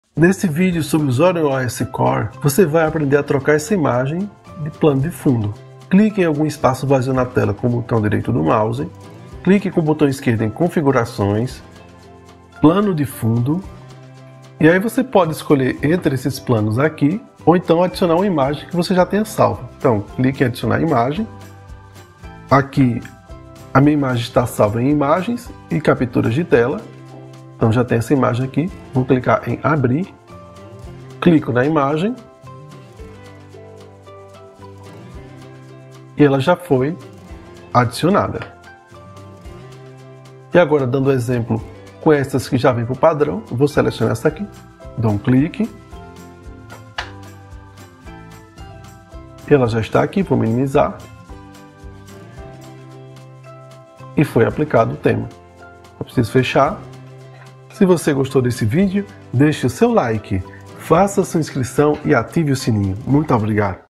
ZorinOS Core - Troque a imagem de fundo da tela inicial
Troque a imagem de fundo da tela inicial do ZorinOS Core.